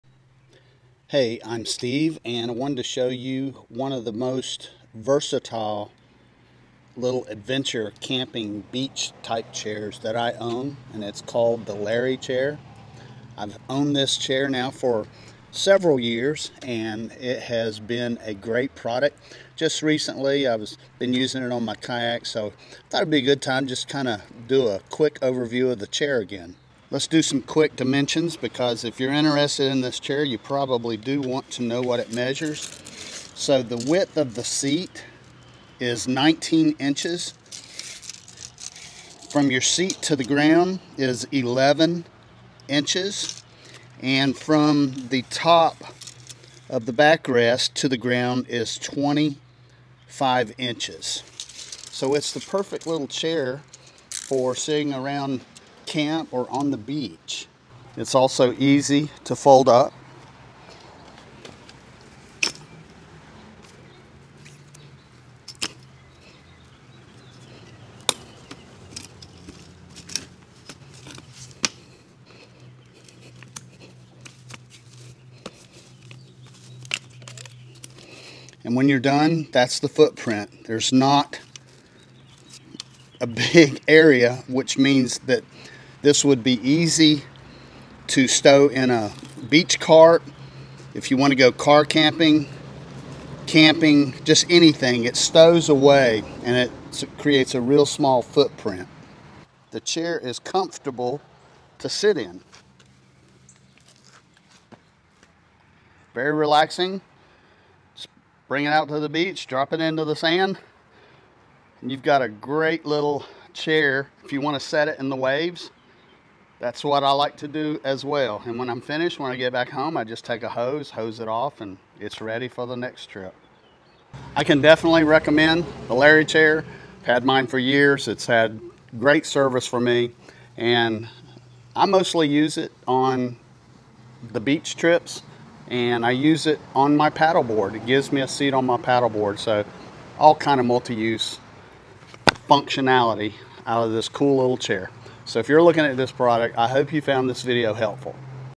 🌟 Introducing the Larry Chair: Your Ultimate Adventure Chair Companion! 🌟
Hey there, adventurers! 🚀 Steve here, and I'm thrilled to share with you one of the most versatile, compact, and adventure-ready chairs in my collection—the Larry Chair! 🪑 I've been a proud owner of this fantastic piece for several years, and let me tell you, it has proven to be a game-changer for all my outdoor escapades, from camping to beach lounging.

📏 Let's dive into the specs, because if you're considering the Larry Chair, you'll want the details. The seat boasts a comfortable 19-inch width, with a ground-to-seat height of 11 inches. The backrest stands at a perfect 25 inches from the ground, ensuring a comfy and supportive experience. Ideal for those chill moments around the campfire or soaking up the sun on the beach!

But what sets the Larry Chair apart is its convenience. The easy fold-up design means you can pack it away effortlessly, and check out the footprint—it's minimal! Perfect for tossing into a beach cart or stowing away in the trunk for your next camping adventure. 🌲

Did I mention its beach-friendly qualities? Simply drop it into the sand, and voila! You've got yourself a cozy spot right by the waves. 🌊 And cleaning up after? A breeze! A quick hose down, and it's as good as new, ready for your next excursion.

Now, let me share a little secret—I've not only used this gem during beach trips, but it's also my go-to seat on the paddleboard! 🏄‍♂️ Yes, you heard that right—multi-use functionality at its finest. The Larry Chair has been my trusty sidekick for years, offering comfort and reliability wherever my adventures take me.

In conclusion, if you're in the market for a travel-friendly, foldable chair that's as versatile as it is comfortable, the Larry Chair is your answer. I've put it to the test, and it's never let me down.

Thanks for tuning in, adventurers! Until next time, stay comfortable and keep exploring! 🗺️